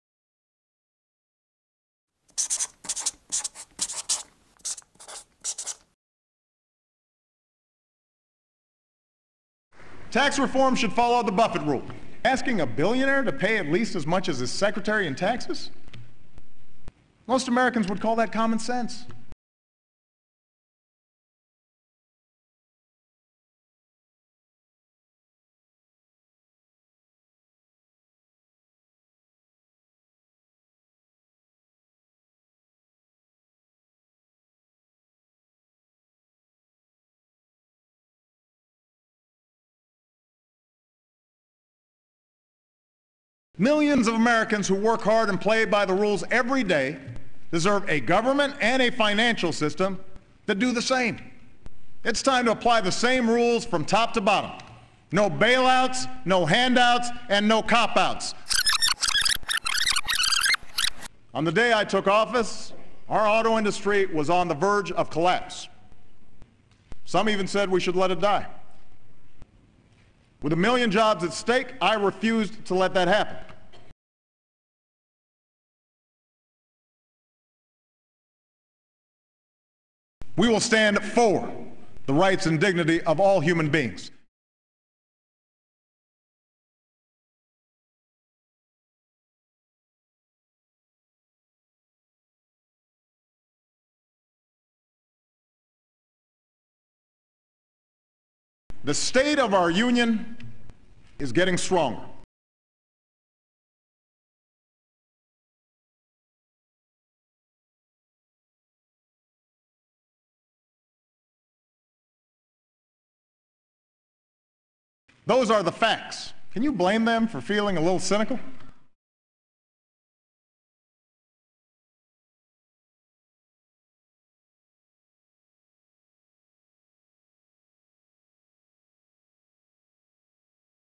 Obama's MIS-State-MENTS of the Union
A few of Obama's errors, lies and mistakes during his 2012 Presidential State of the Union Address.